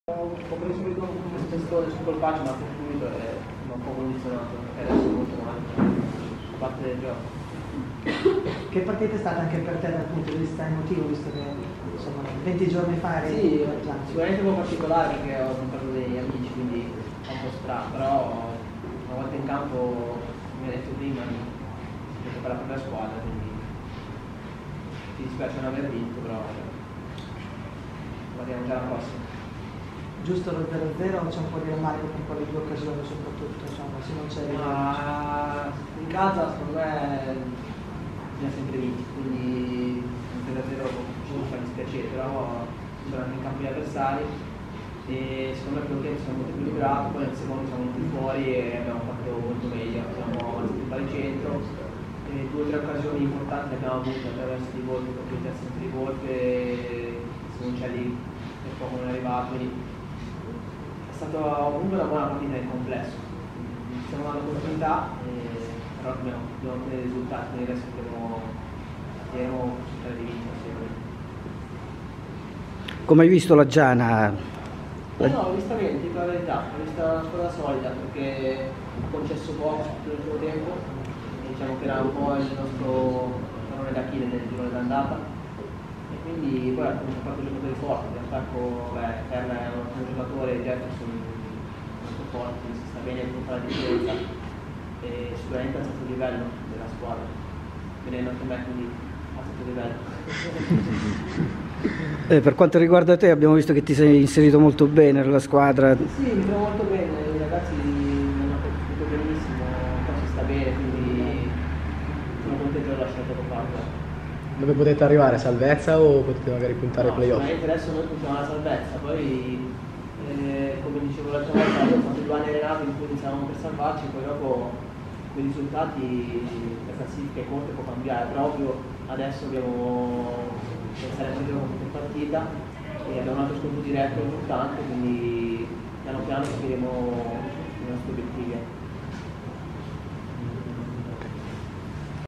Antonio Palma Rimini Giana Erminio 0-0
Le dichiarazioni dell'ex Biancazzurro Totò Palma dopo il pari tra Rimini e Giana.